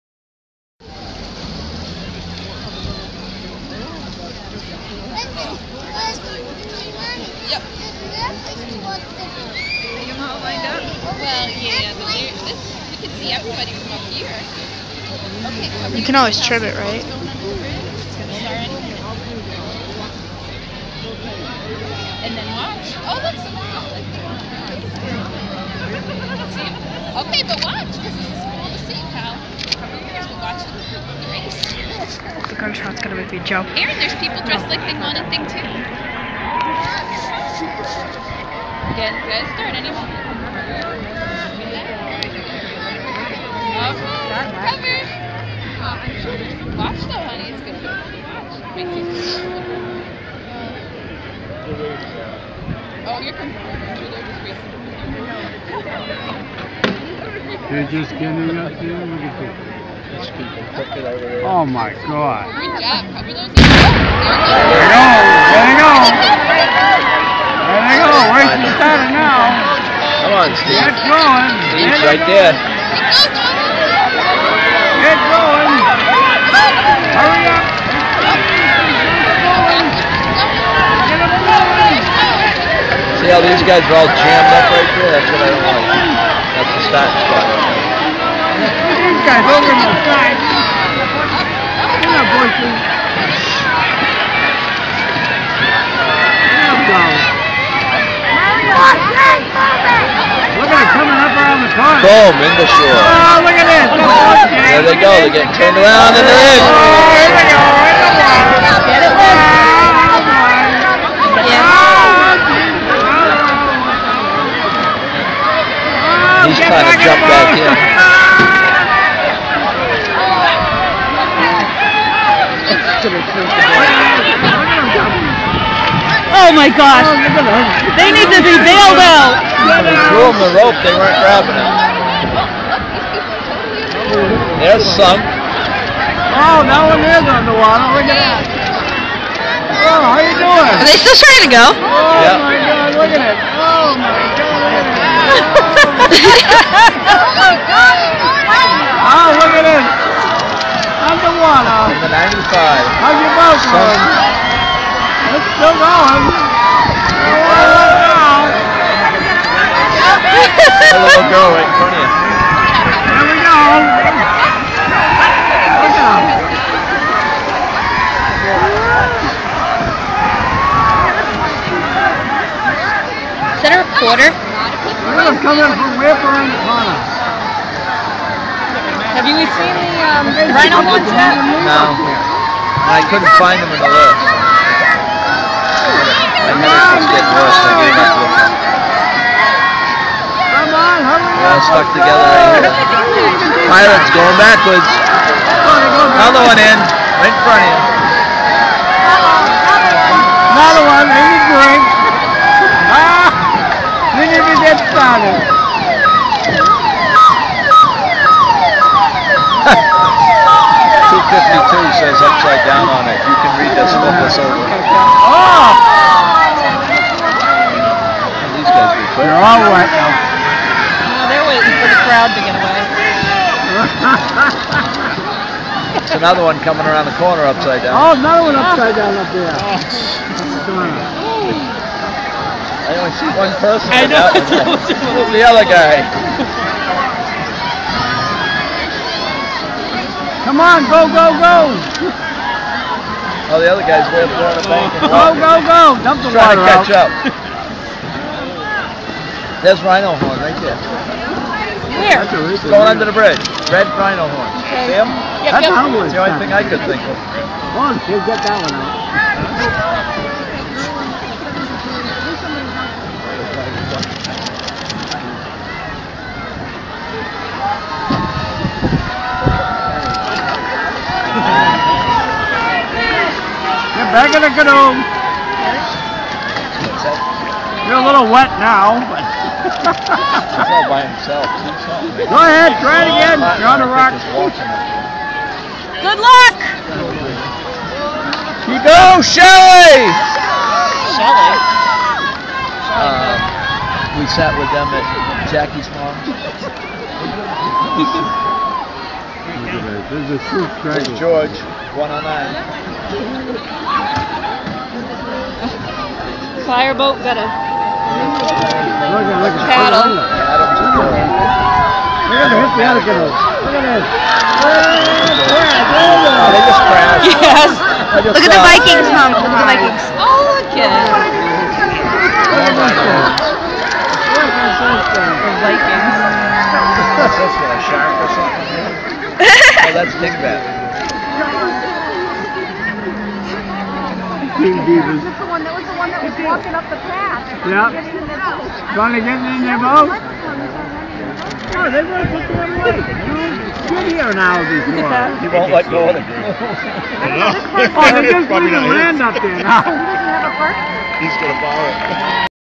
2013 River Rat Race Begins
Paddlers pass under the South Main Street Bridge at the start of the 2013 River Rat Race.